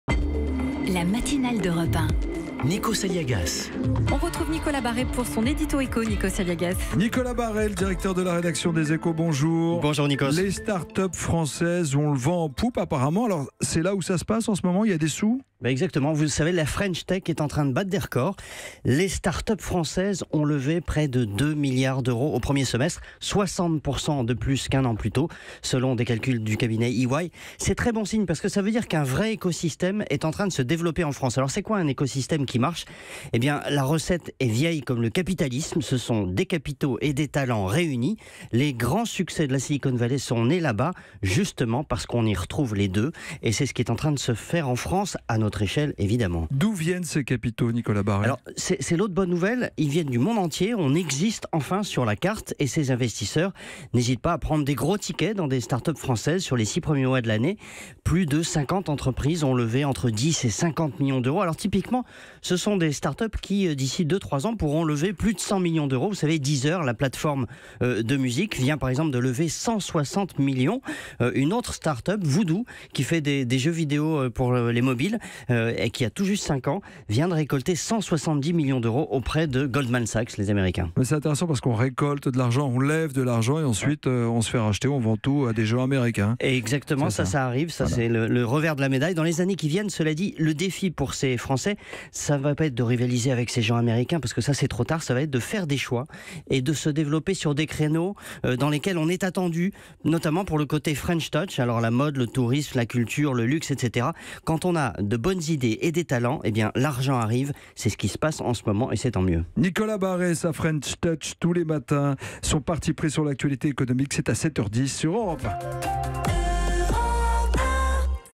Les starts-up n’ont jamais levé autant de fonds !
Chaque matin, Nicolas Barré fait le point sur une question d'actualité économique.